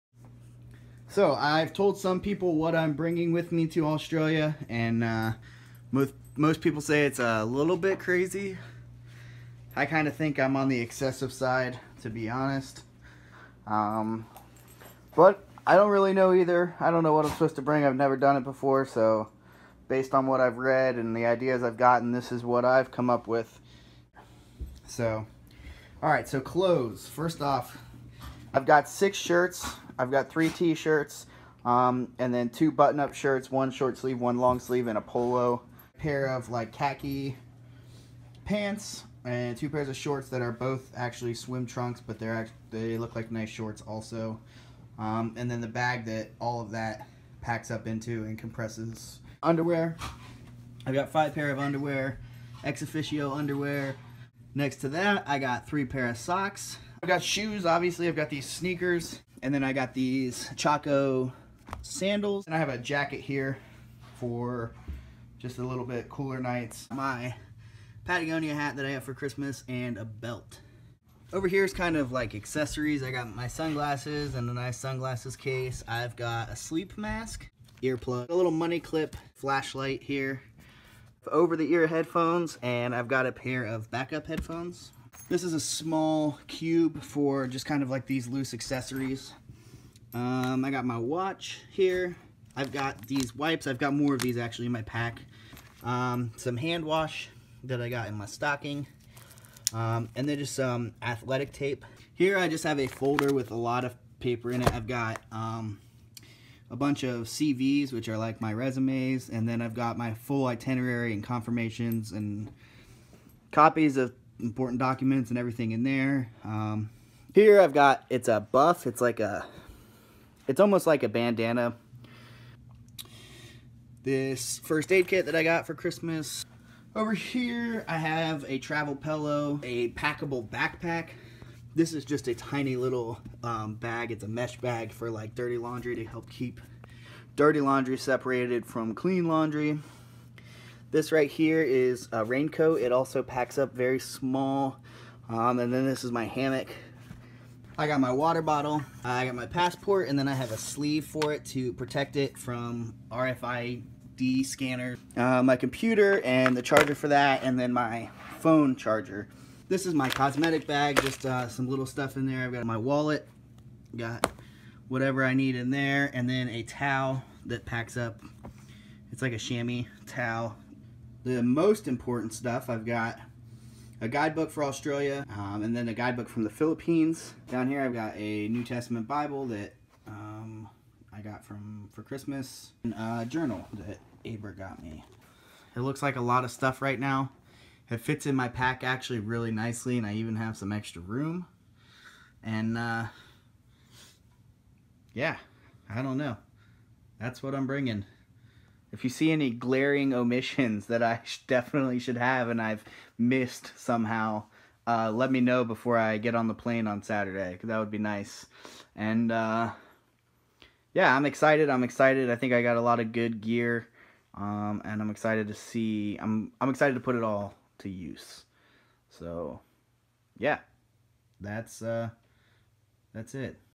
Whats in my backpack
This is a quick video of everything thats in my backpack.